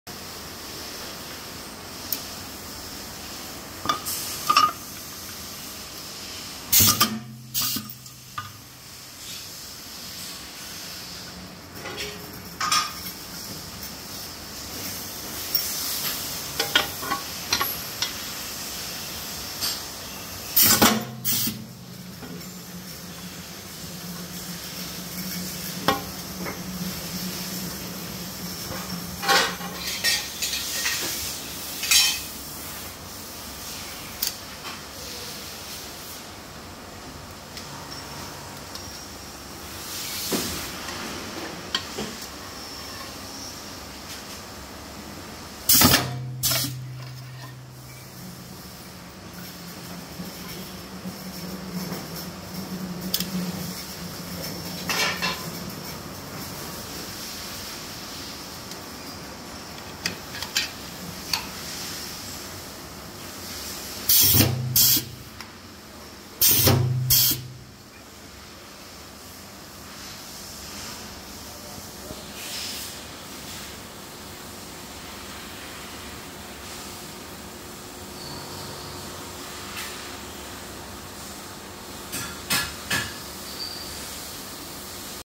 Mini Shoham - The best of both worlds (3 manual units into 1 pneumatic)
בסרטון 3 ידניים במכבש פניאומטי 1:
• פינוי שתל מזוזה לקבוע 7000
• פינוי שתל מזוזה לקבוע 9000
• פינוי קבוע אינטגרלי 7000
לדוגמה: מחיר מיוחד לשילוב הנ"ל 7000 ₪
ניתן להזמין אין סוף שילובים לפי בחירתכם:
• חור רשת קלה
• פינת ארגז
• רצועת ארגז
• פינת מתיחה חיצונית 4500
• פינה טרודה 4400
• פינוי שתל מזוזה לקבוע 9000 / 7000 / 1700
• חורי מזוזה 4/5 נתיבים
• עוד ועוד..